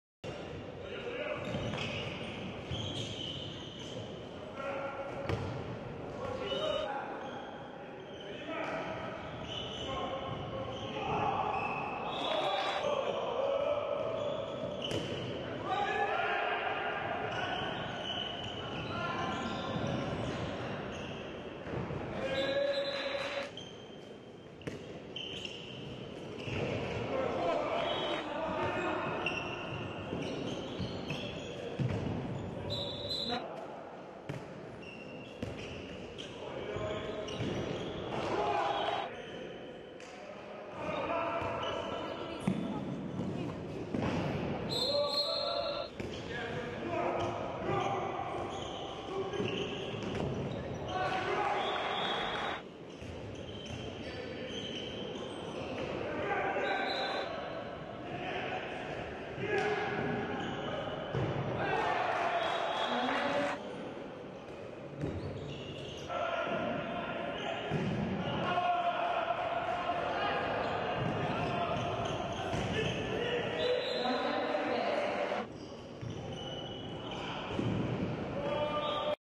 Гандбол Суперлига 2016/2017. ЗНТУ-ЗАС 25:33 Мотор. Запорожье, 18/01/2017. Моменты матча
Гандбол Суперлига 2016/2017. ЗНТУ-ЗАС  25:33 Мотор. Запорожье.